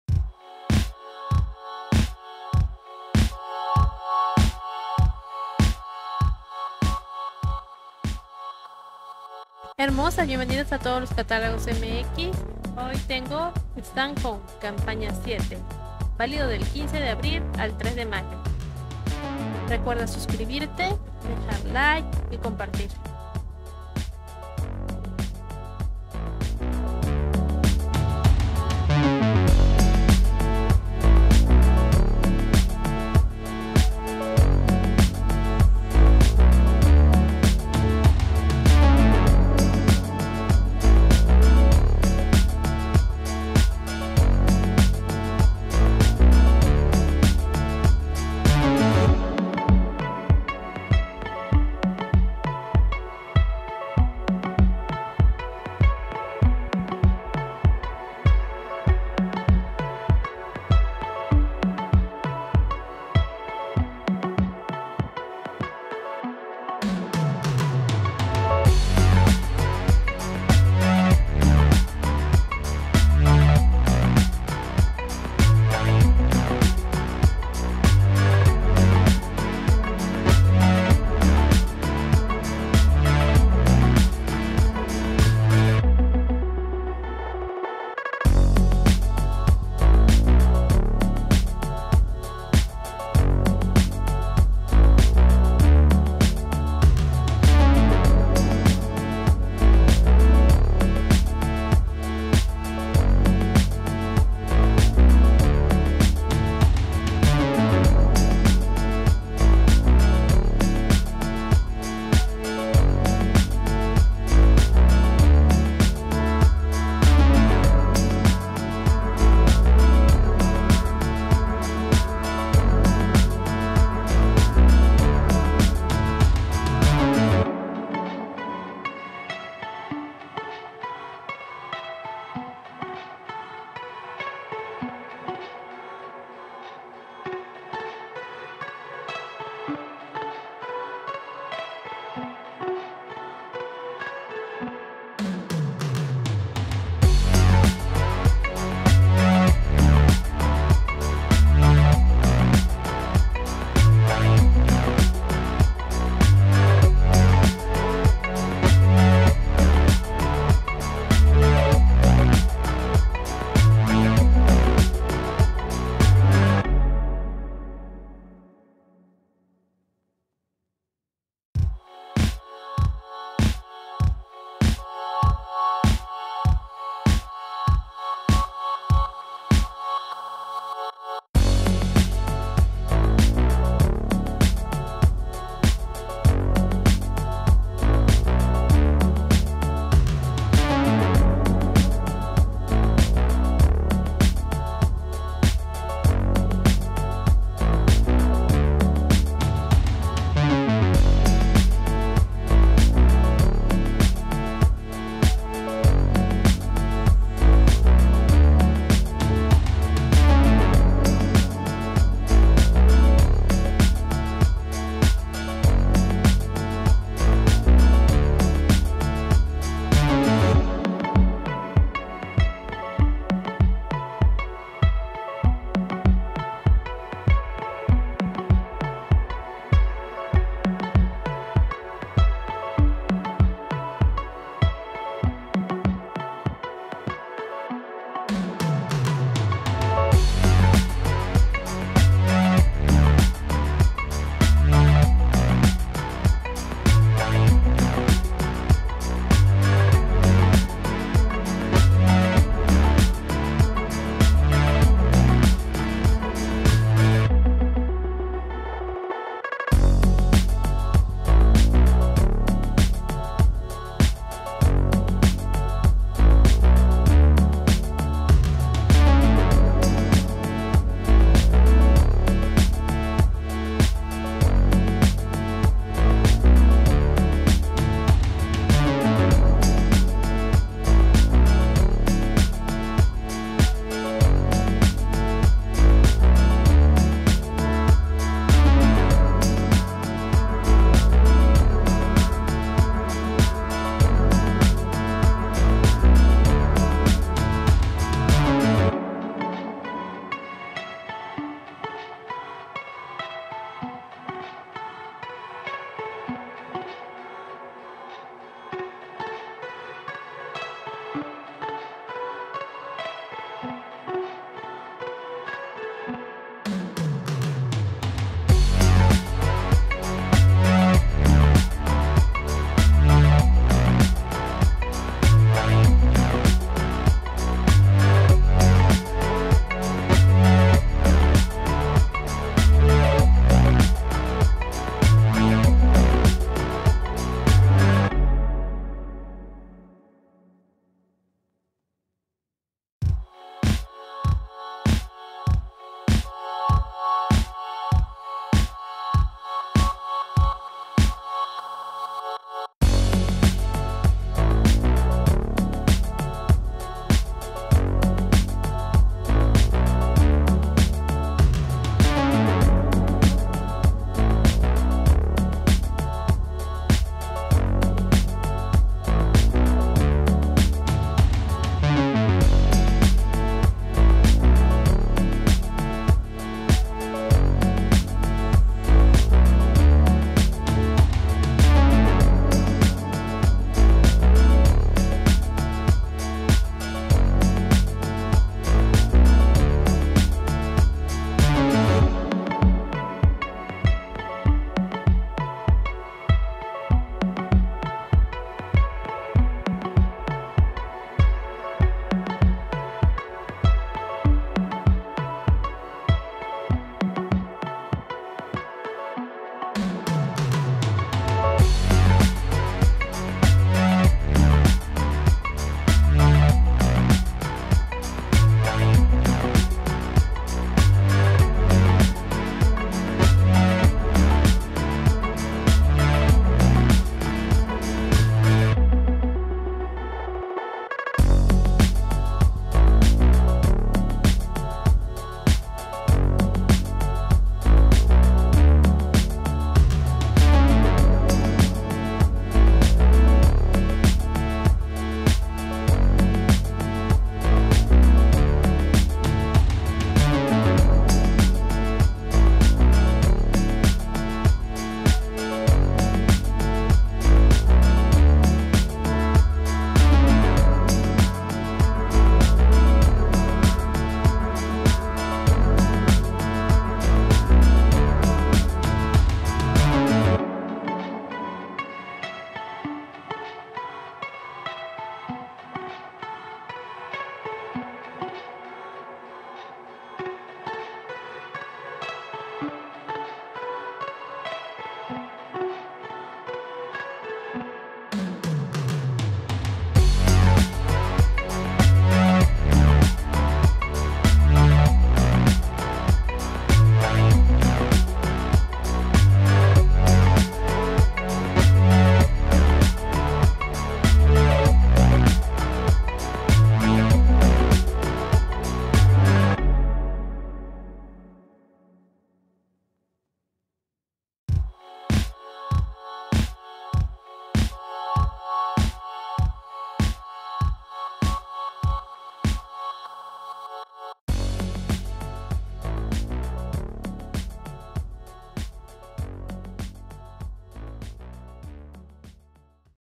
STANHOME Campaña 07 2024 Valido del 15 de abril al 3 de mayo 2024
🤩Hola Hermos@s hoy traemos para ti, el catalago STANHOME Campaña 07  2024
💠 Valido del 15 de abril al 3 de mayo 2024